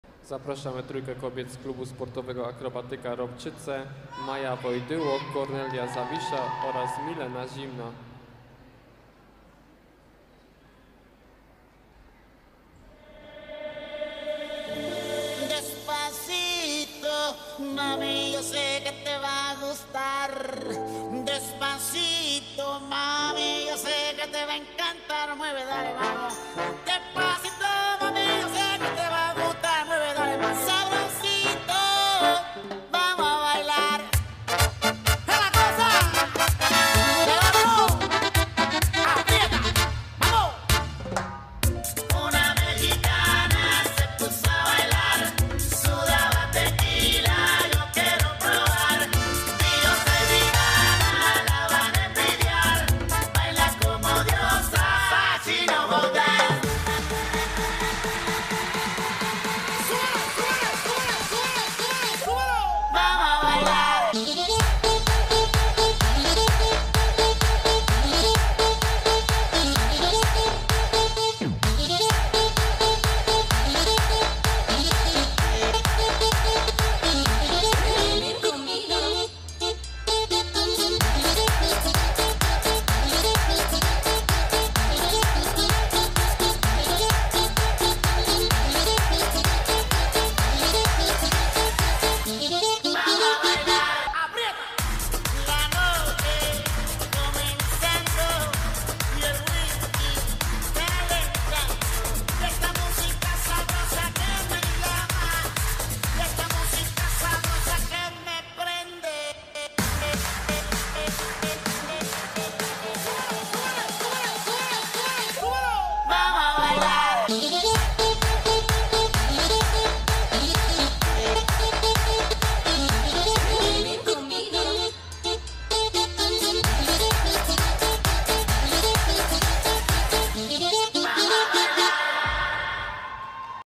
Mistrzostwa Polski Juniora Młodszego Chorzów
WOJDYŁO Maja
ZAWISZA Kornelia
ZIMNA Milena
Klub Sportowy Akrobatyka Ropczyce
3K-II Statyka- eliminacje